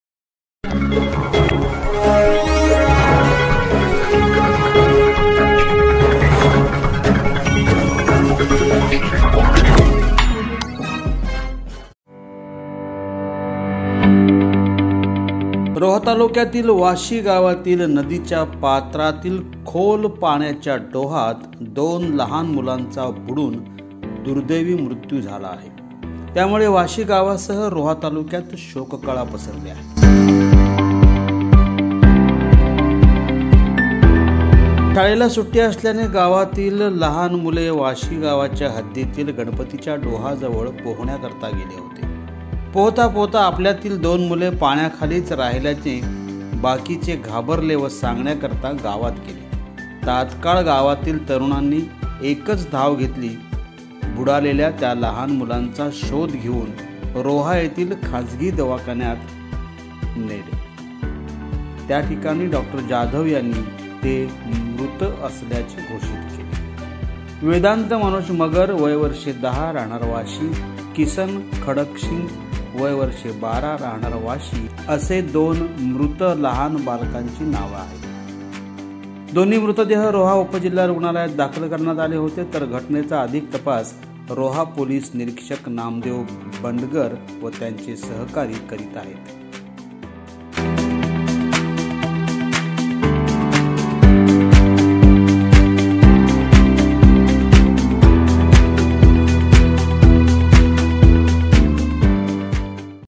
#२७ फेब्रु २०२१
#२७ फेब्रु २०२१ रोह्यात_दोन_बालकांचा नदीत_बुडून_मृत्यू...! रोह्यात शोककळा... पहा पत्रकार दीपक शिंदे यांचे *आपलं विश्व*